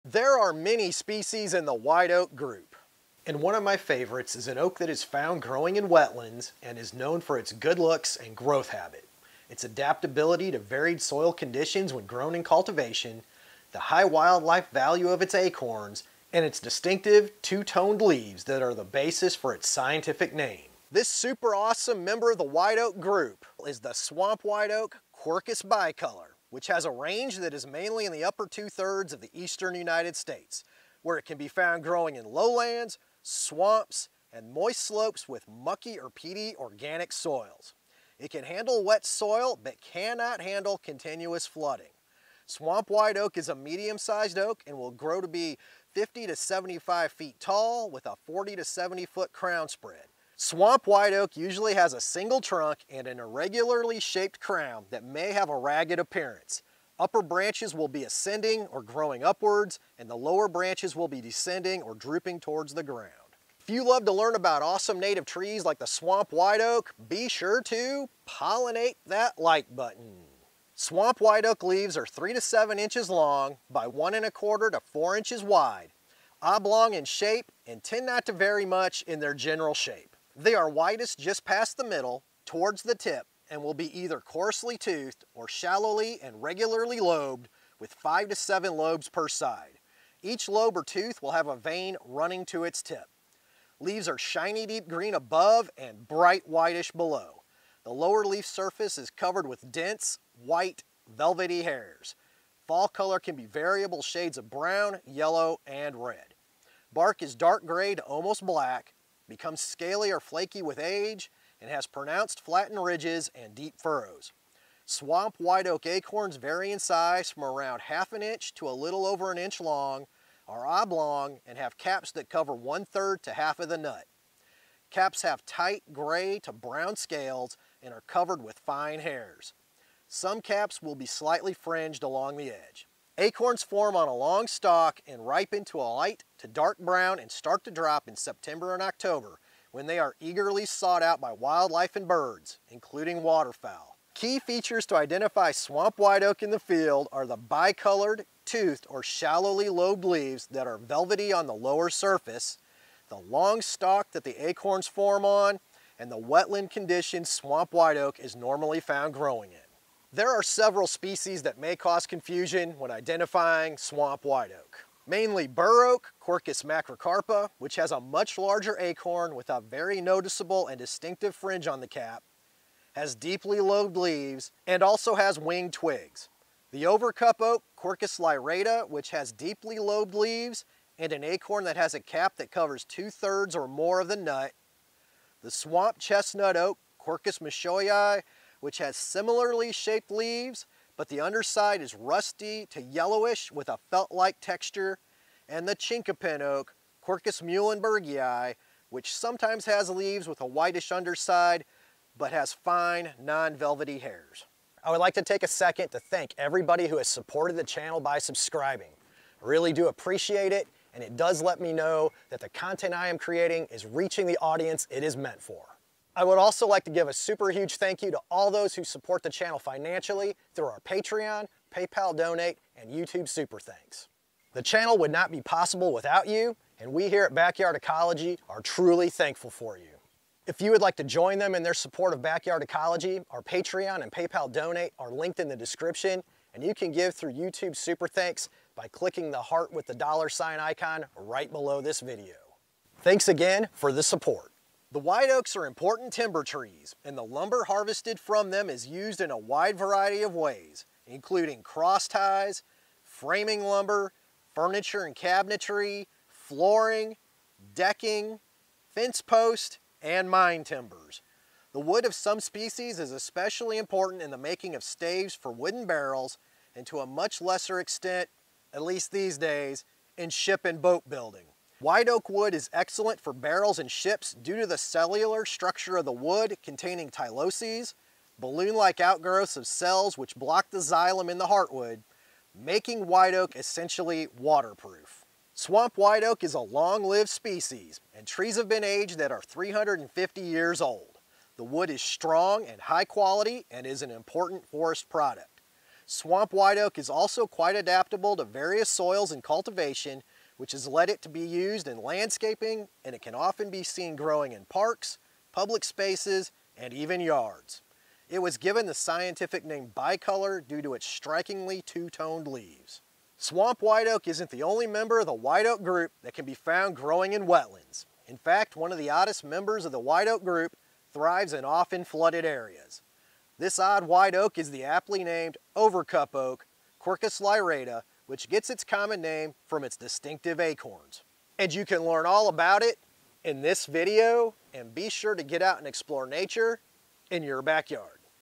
Discover the SECRET to Identifying Swamp White Oak FAST!
🌳🌳🌳🌳  Learn about another wetland white oak species, the overcup oak in this video:  🌳🌳🌳🌳

The swamp white oak, Quercus bicolor, is an important member of the white oak group that can be found growing in moist soils across much of the eastern United States. Although less known than some species in the white oak group, swamp white oak produces acorns that feed a variety of wildlife, has wood that is considered high quality, and is even well adapted to being used as a landscape tree outside of the wetland habitat it naturally occurs in. Learn how to identify swamp white oak and tell it apart from similar species as well as some interesting facts about it in this video!

Good Old Snail Mail:
Grassy Roads LLC

Chapters:
0:00 There Are Many White Oaks
0:23 Swamp White Oak, Quercus bicolor, Native Range, Typical Growing Conditions, Size, Growth Form
1:10 Swamp White Oak, Quercus bicolor, Identification by Leaf Characteristics, Leaf Size, Leaf Shape, Leaf Color, Fall Color
1:47 Swamp White Oak, Quercus bicolor, Identification by Bark Characteristics, Bark Color, Bark Ridges and Furrows, Bark Scaly and Flaking
1:54 Swamp White Oak, Quercus bicolor, Identification by Acorns, Acorn Size, Acorn Color, Acorn Maturity and Drop, Acorn Cap,  Acorn Use by Wildlife
2:25 Swamp White Oak, Quercus bicolor, Key Features for Identification
2:40 Species that Might Be Confused with Swamp White Oak, Bur Oak, Quercus macrocarpa, Overcup Oak, Quercus lyrata, Swamp Chestnut Oak, Quercus michauxii, Chinkapin Oak, Quercus muhlenbergii
3:25 Thank You for Supporting Backyard Ecology and How You can Help Support the Channel
4:05 Uses for White Oak Wood and Lumber, Barrels, Ships, Lumber, Cross Ties, White Oak Wood Tyloses
4:46 Swamp White Oak, Quercus bicolor, Use as a Shade and Specimen Tree, Longevity, Lumber Importance and Quality, How Swamp White Oak Got Its Scientific Name
5:14 Swamp White Oak Isn’t the Only White Oak to Live in Wet Areas, Overcup Oak, Quercus lyrata